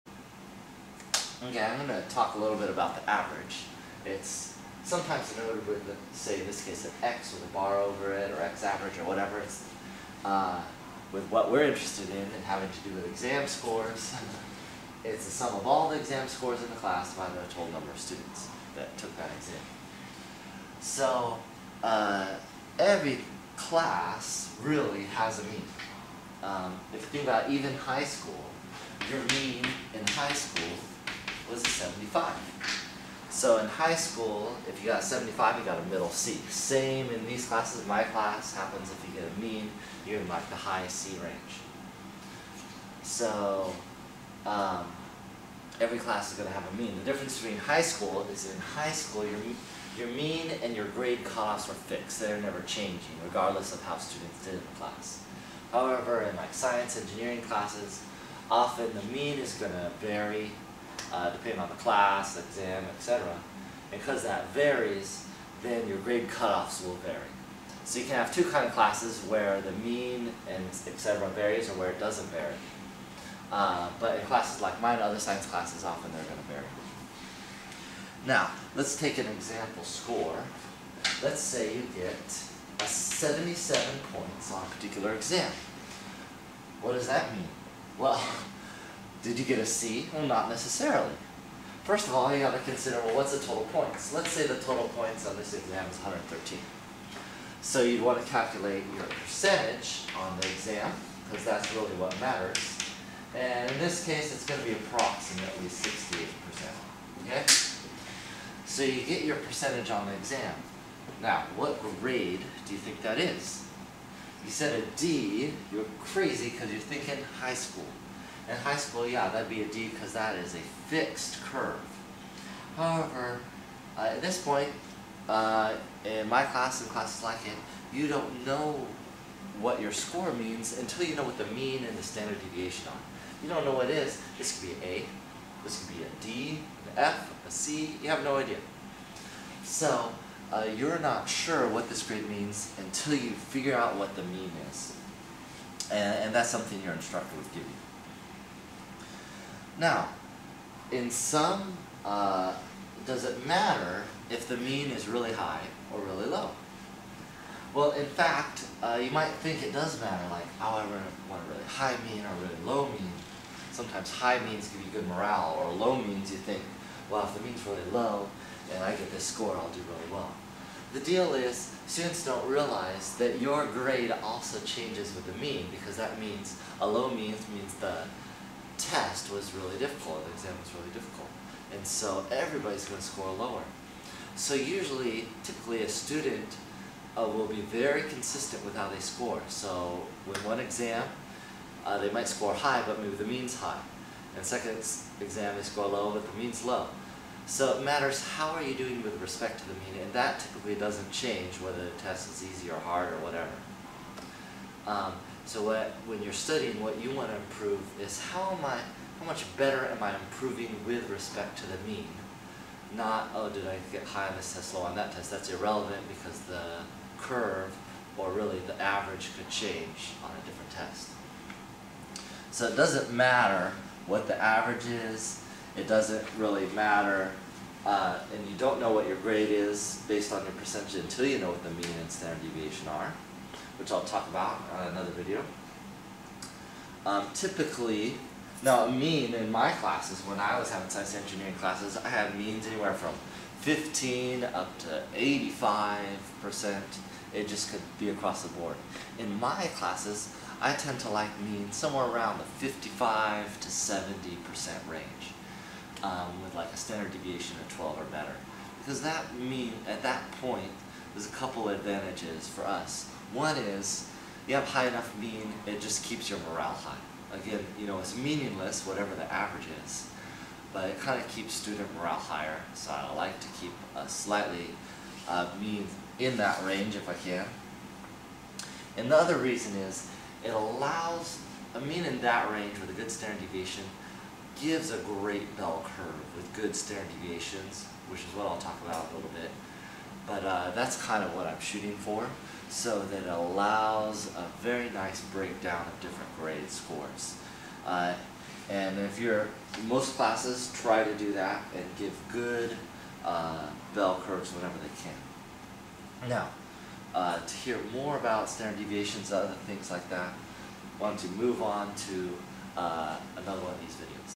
Mean Average.mpg
How does the mean (average) work?  In this case, we'll look at grades on an exam.